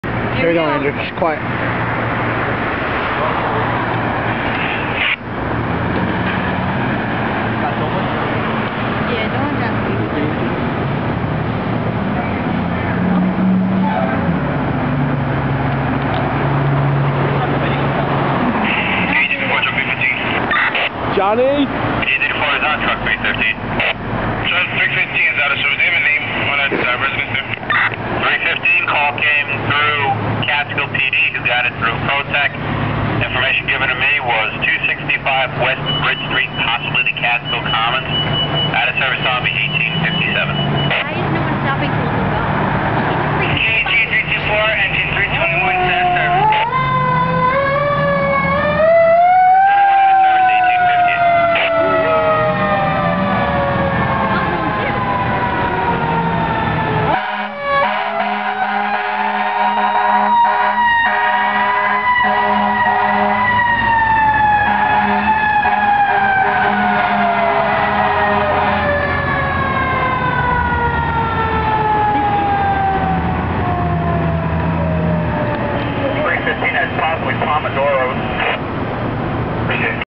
Fire Engines Responding
fire units responding to an alarm drop.

This video is property of Echo-Delta-Niner photography